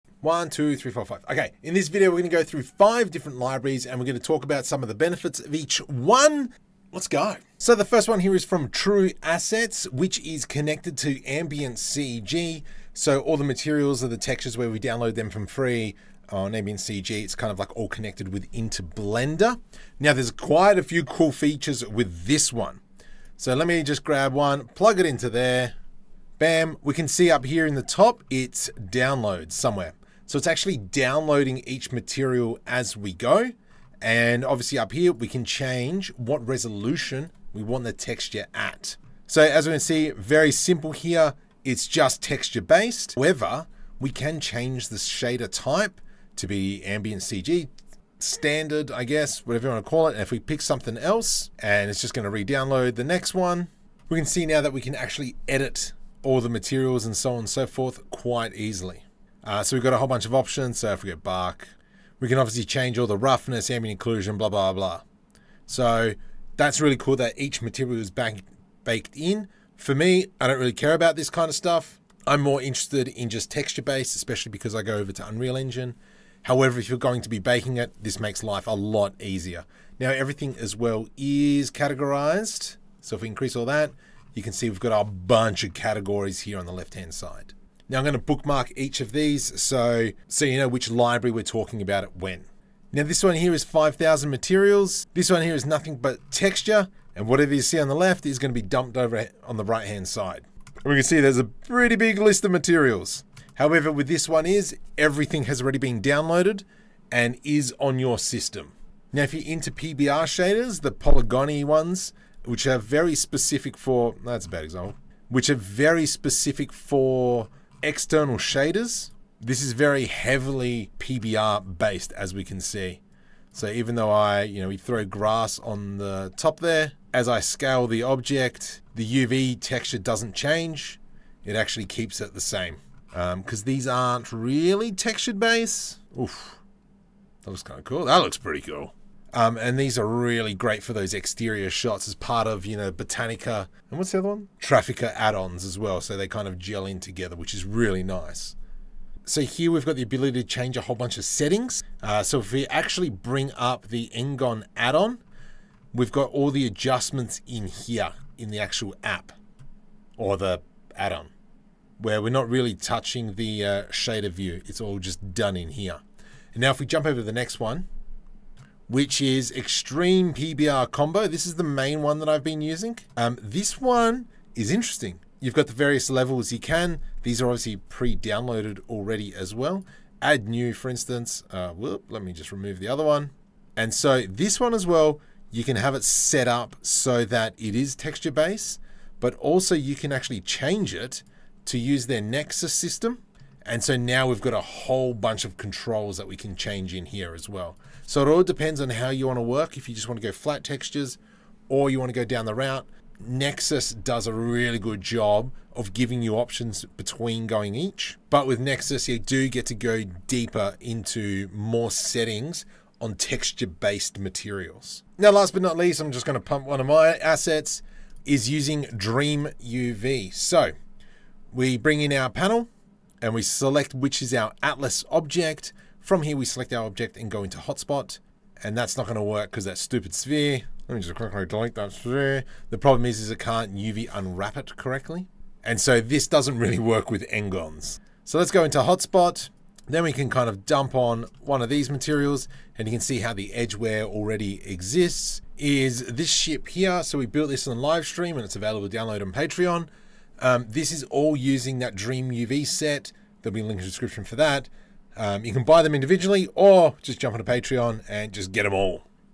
Discover 5 Material Libraries in Blender
Looking for new materials for your Blender projects? Check out these 5 material libraries that you can download and use in your designs! Improve your workflow with these awesome resources.

00:00 Intro
00:07 True Assets
01:32 5000 Materials
01:48 Materialiq
02:48 ExtremePBR Nexus
03:35 DreamUV Maps